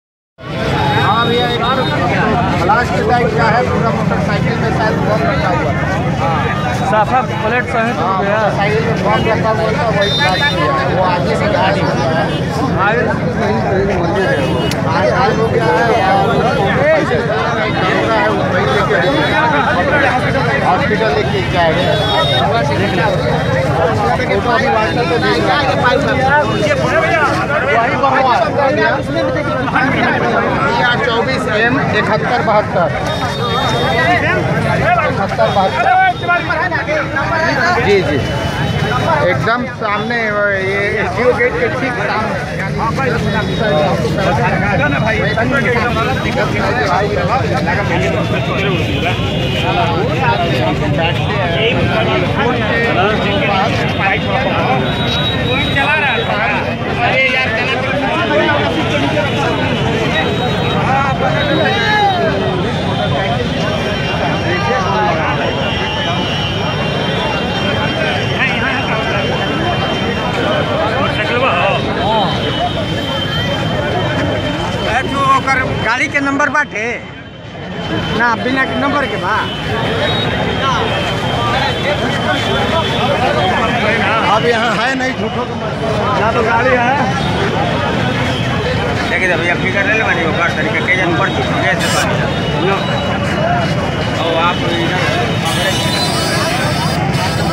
Bihar Sasaram Bomb blast outside Court
सासाराम व्यवहार न्यायालय परिसर के बाहर आज फिर बम ब्लास्ट हुआ, जिसमें एक की मौत हो गई है और एक पुलिसकर्मी समेत दो लोग घायल हो गए।